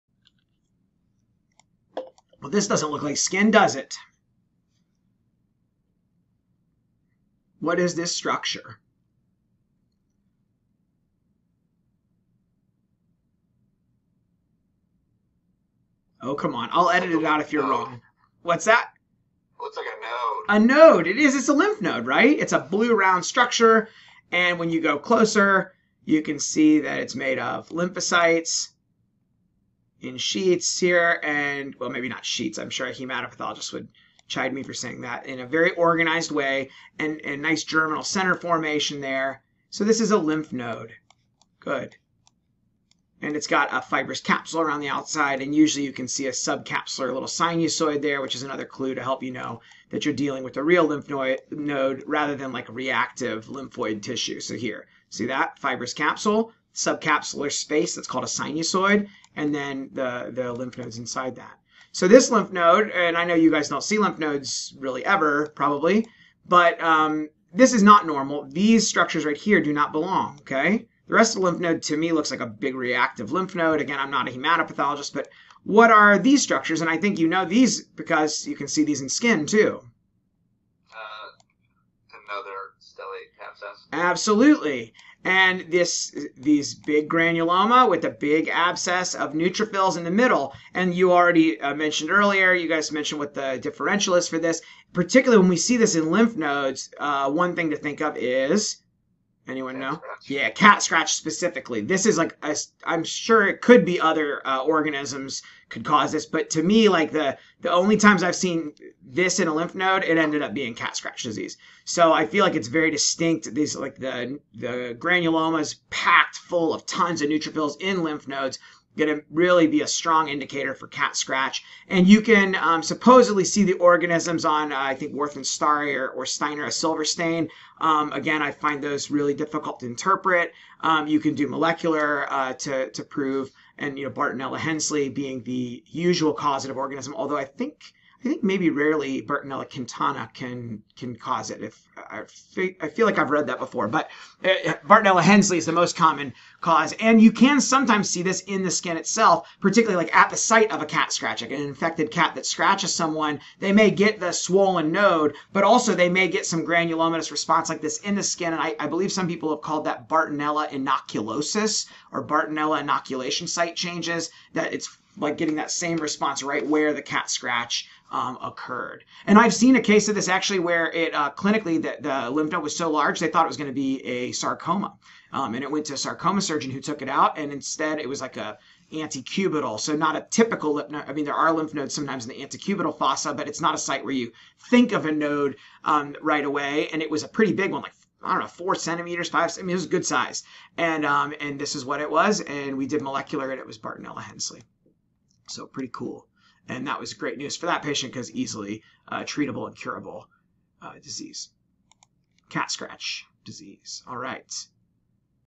Cat Scratch Disease (Bartonella henselae): 5-Minute Pathology Pearls
Excerpt from my infectious dermpath board review video (20 classic bacteria and fungus dermpath cases in 1.25 hours).

Virtual slides courtesy of my friend and colleague, the amazing Dr. Tammie Ferringer (the platform they are hosted on doesn't have the ability for public sharing of the WSI files...sorry!)